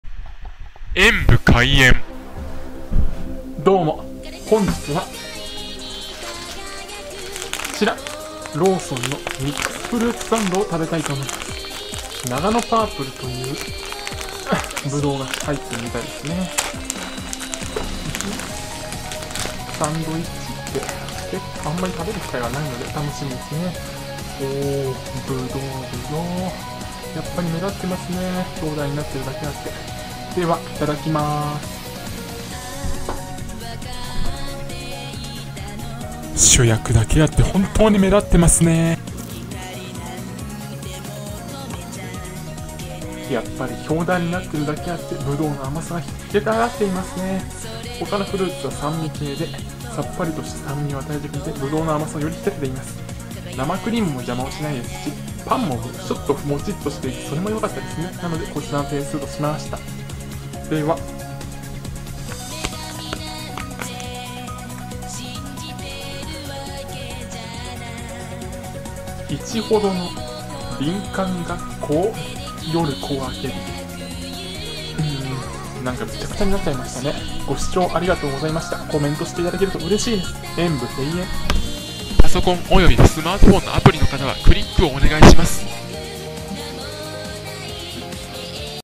ローソン フルーツサンドナガノパープルを食べてみた。葡萄が本当に目立っています！！
ローソン　フルーツサンドナガノパープルを食べてみました。
表題になっているだけあって目立っていますね！！
贈り物は此方まで↓
〒153-0042